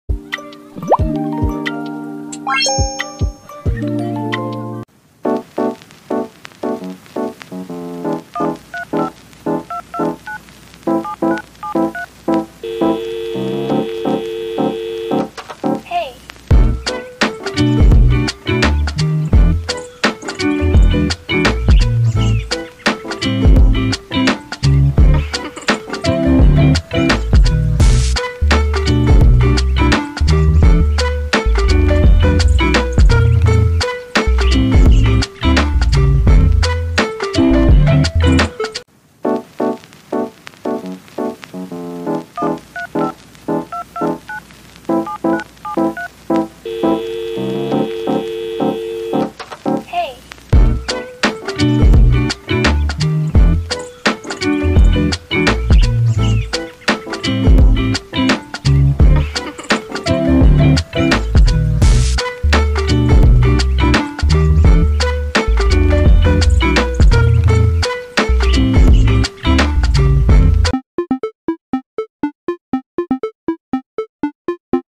3d Digital Calligraphy || angelic_.arts
App used: Ibis PaintX
Style used credit to Alyza Jean Datuin

•Other infos:
3d pen (custom brush)
Felt Tip Pen (hard)
Nikoru pen (custom brush)
f(x) filter: drop shadow

angelic_.arts